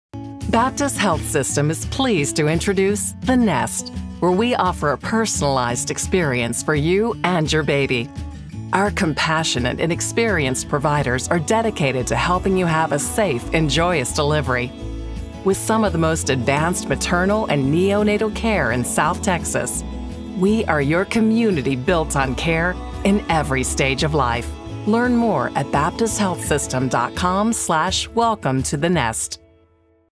The Nest – Women's Services at Baptist Health System
While every birth at Baptist Health System's The Nest is unique, they all share one thing in common: our staff’s dedication to providing the best compassionate care possible for moms, their babies, and their families. Everything you need for you and your baby is right here, including care for high-risk pregnancies and access to Level III and IV Neonatal Intensive Care Units (NICU).